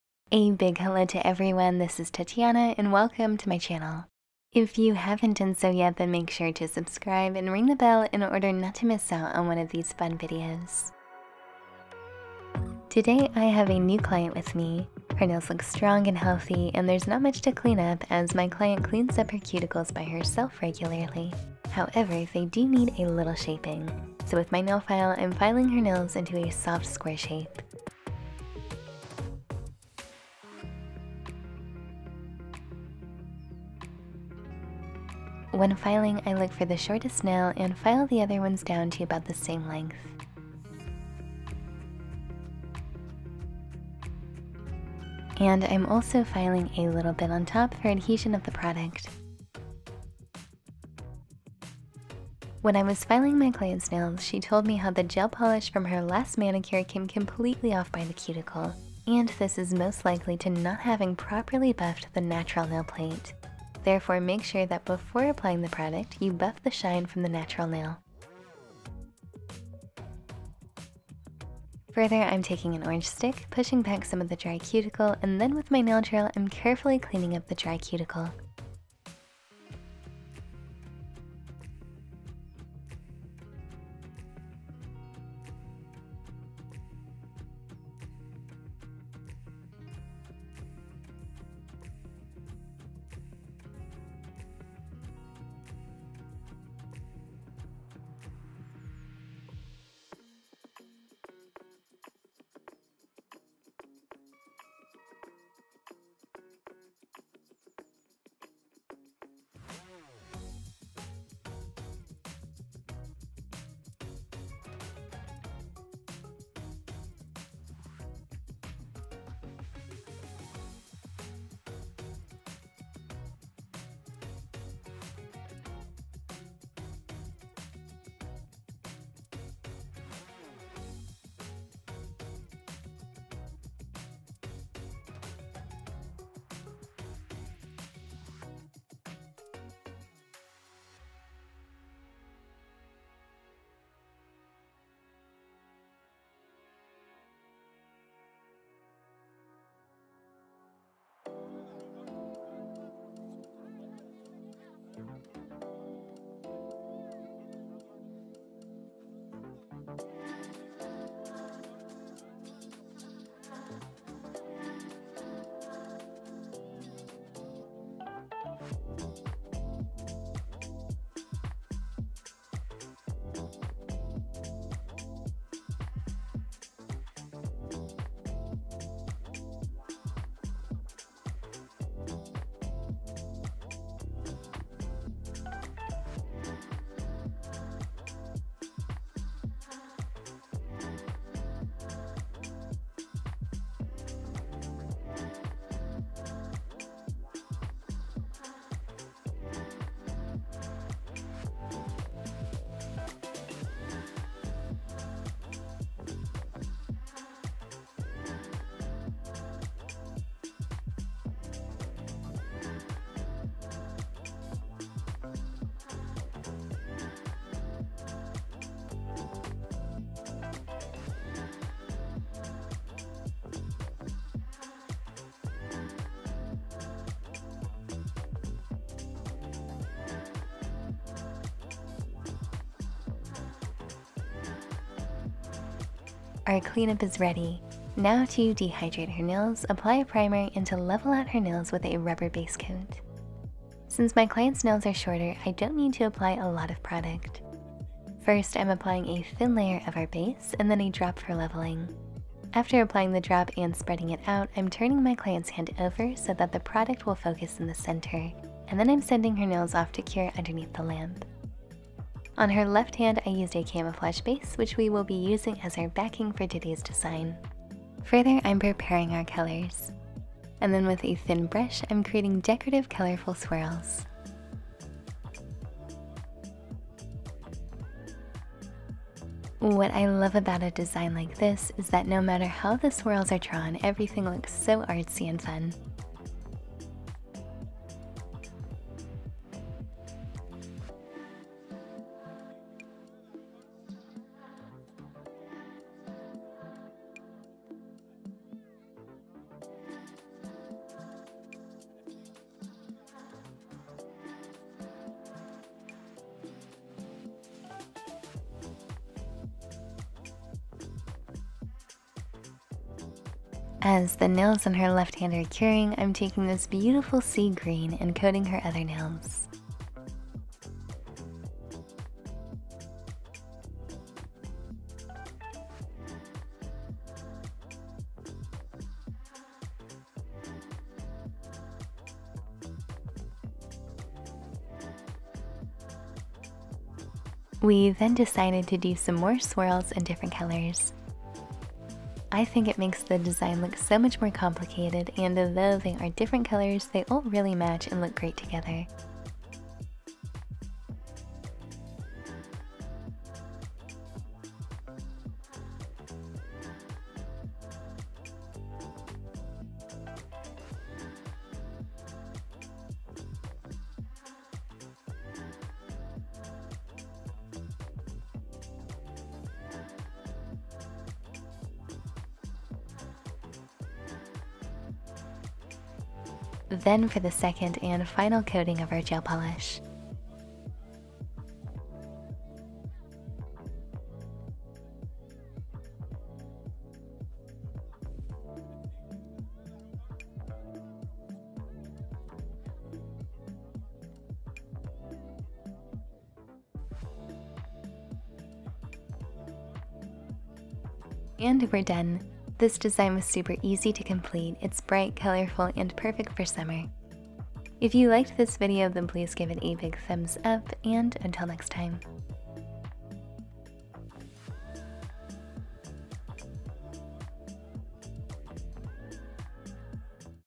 Manicure On Short Natural Nails, Simple Colorful Design
I love the way this set turned out! Colorful and fun for summer

ℹ️ What I use:
Video voiced over by Catherine
What I film with:

Main - Sony Sony A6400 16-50 mm
Side shots - Sony A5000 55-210 mm